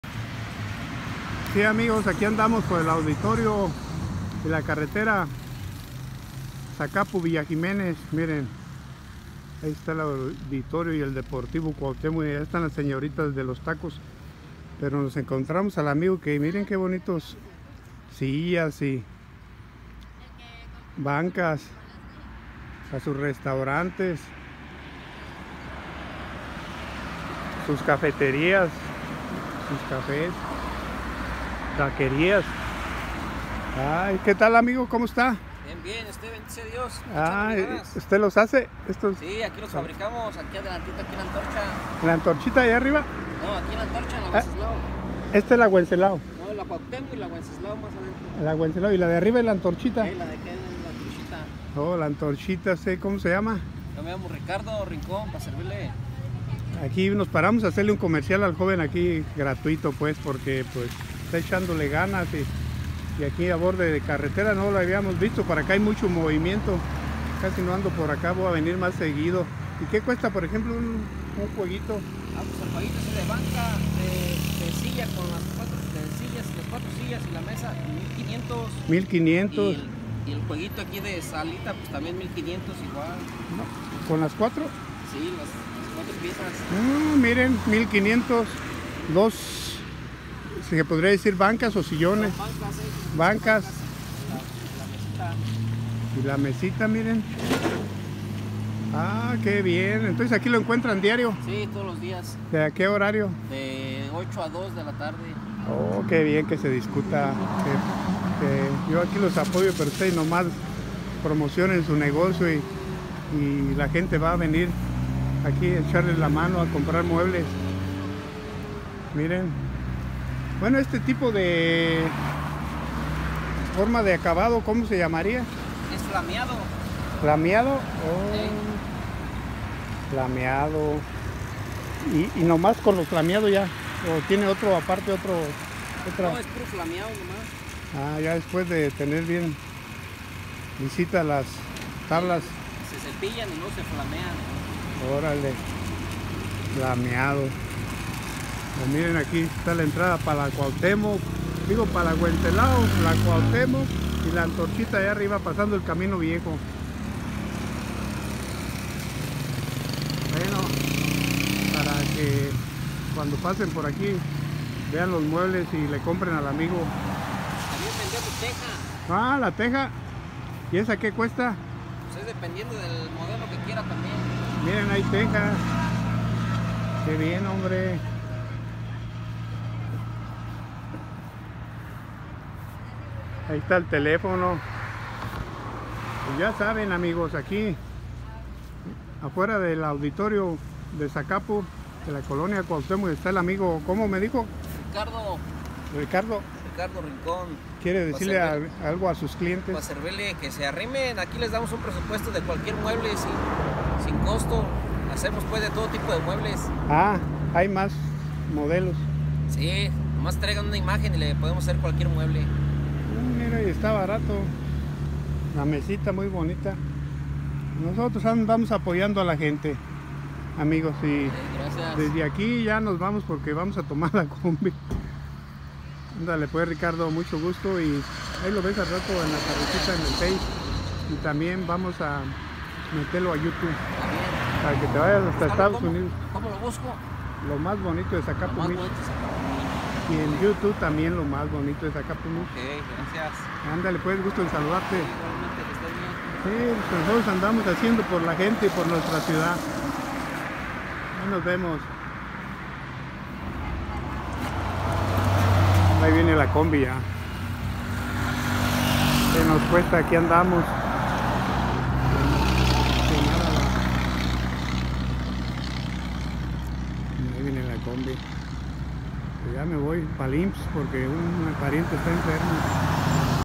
🤩✨💫🇲🇽POR EL AUDITORIO ENCONTRAMOS UNAS SILLAS Y BANCAS. MESAS MUY BONITAS DEL JOVEN RICARDO ZACAPU
🤩✨💫🇲🇽POR EL AUDITORIO ENCONTRAMOS UNAS SILLAS Y BANCAS. MESAS MUY BONITAS DEL JOVEN RICARDO MUY BONITAS DE ZACAPU MICHOACAN.